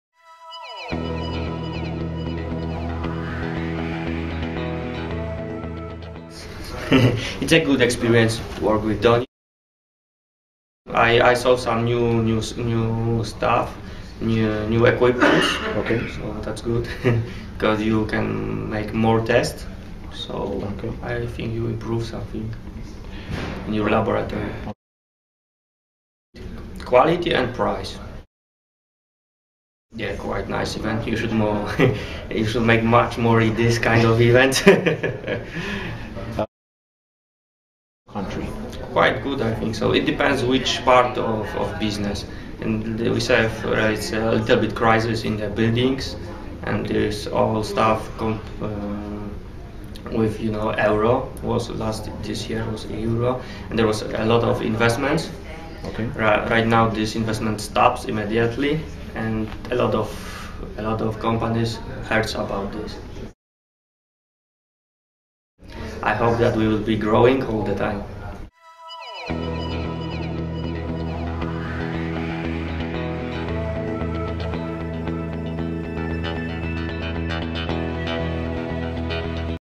Donit Customer Testimonials - Bartloomeij Chlosta. General Manager at ANBI-TECH, Poland.
Mr. Chlosta, Director General at ANBI-TECH Poland explains his experience working with Donit and tell us abou current market situation in Polan.